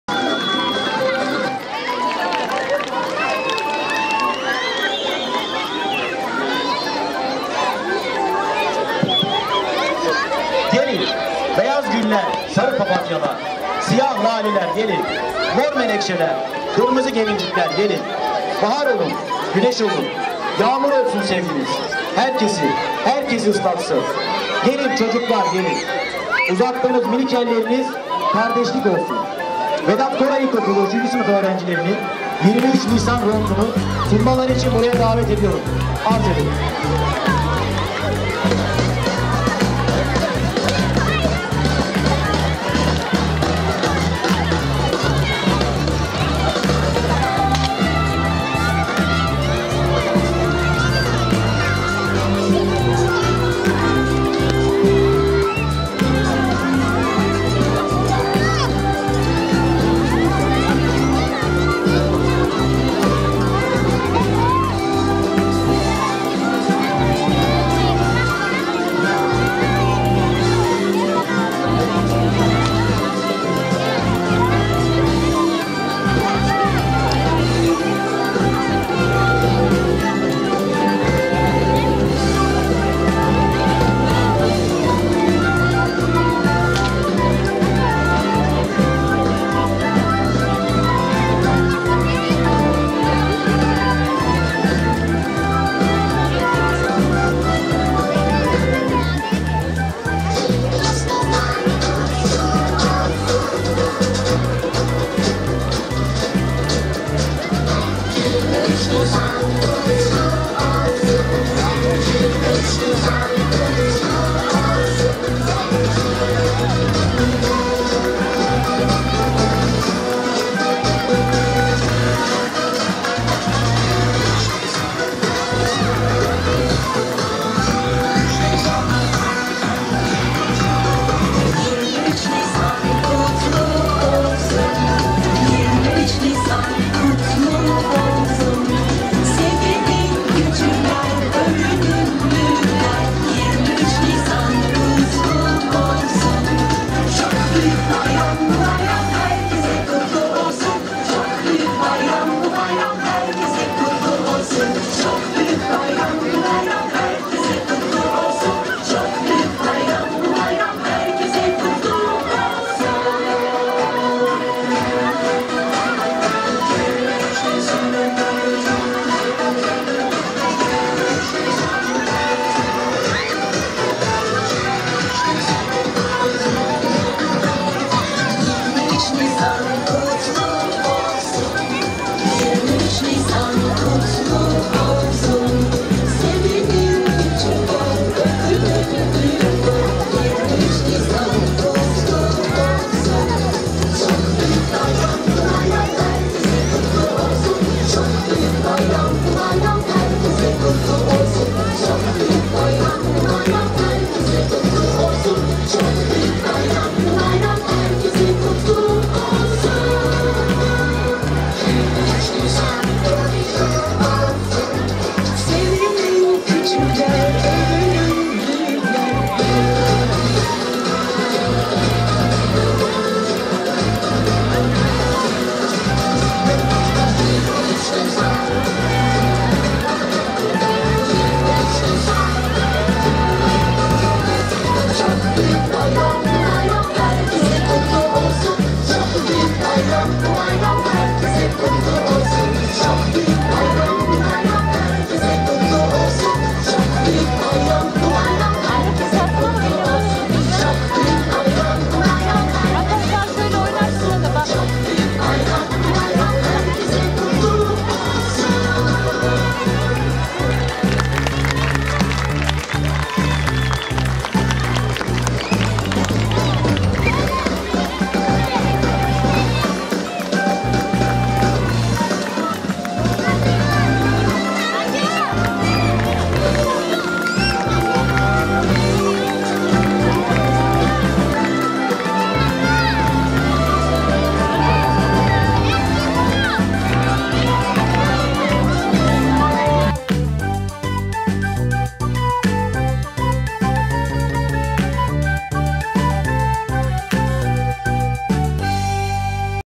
23 Nisan Ront Gösterisi
23 nisan ulusal egemenlik ve çocuk bayramı ront gösterisi. iyi seyirler